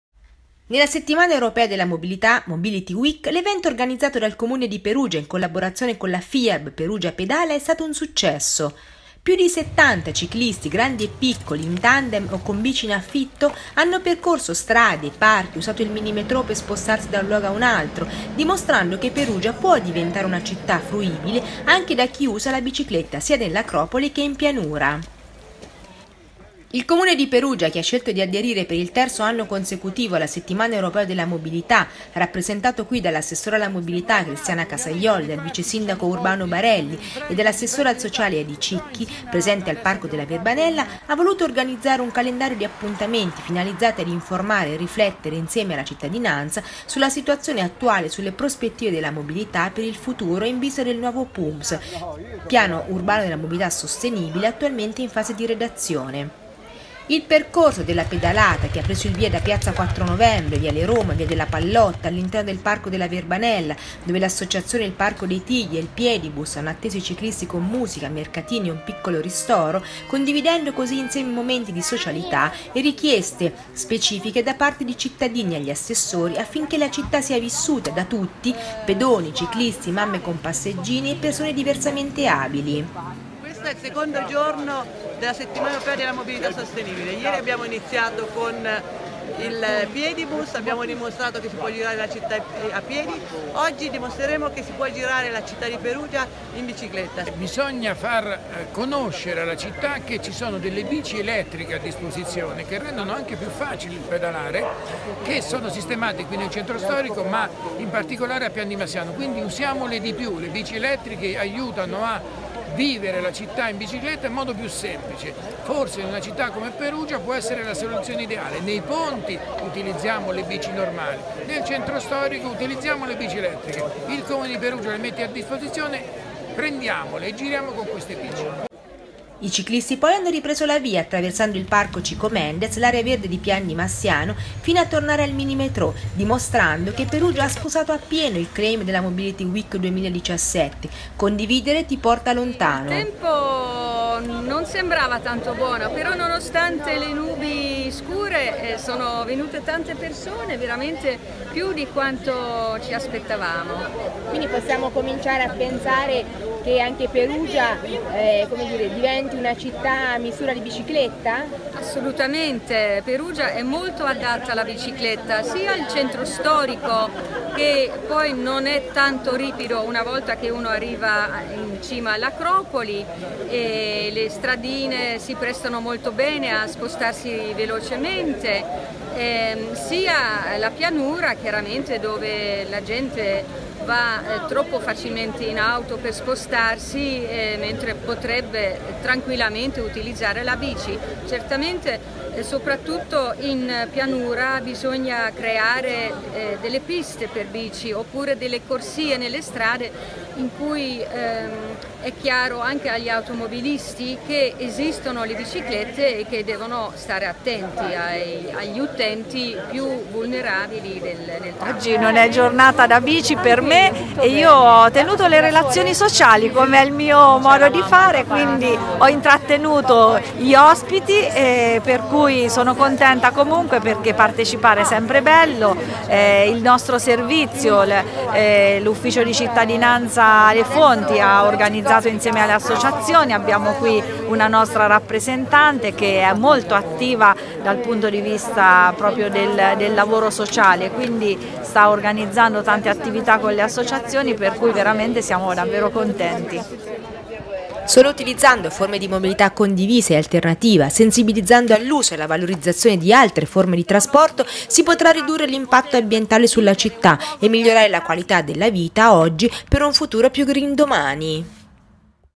partecipa pedalando in città e nei parchi
Si è svolto oggi a Perugia il secondo evento dedicato alla mobilità alternativa nella Settimana Europea della Mobilità. "Perugia in bicicletta, pedoni e ciclisti si incontrano al parco", una pedalata organizzata in collaborazione con la FIAB Perugia Pedala che ha visto la partecipazione di più di 70 ciclisti che partendo da piazza IV Novembre hanno percorso strade, parchi, il minimetrò fino ad arrivare al Parco della Verbanella ed incontrare i pedoni del Piedibus Speciale Quartiere in Festa e le associazioni Il Profumo del Tiglio, l'associazione ADA e La Bottega Ars et Labora. All'evento hanno partecipato l'assessore alla mobilità Cristiana Casaioli, il vice Sindaco Urbano Barelli e l'assessore ai servizi sociali Edi